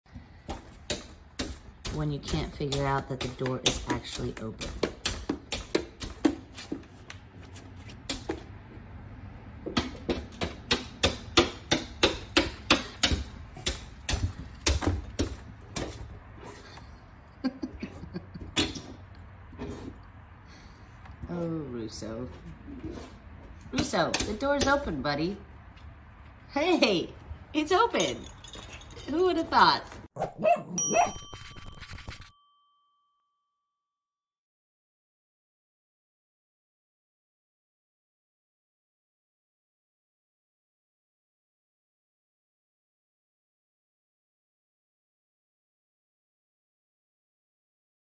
Puppy gets hilariously angry at glass door
Russo gets angry at the door, wanting to go in the house, but doesn’t realize it’s open, and it's such hilarious footage! Check it out!